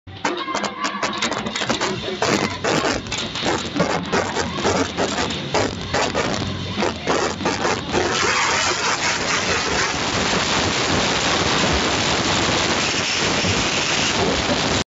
Tor - LLK Kärnten / ECV Tlgers am 13.03.2016 09:14
Tor in der Partie - LLK Kärnten gegen ECV Tlgers beim ÖM U12 : 1A -2B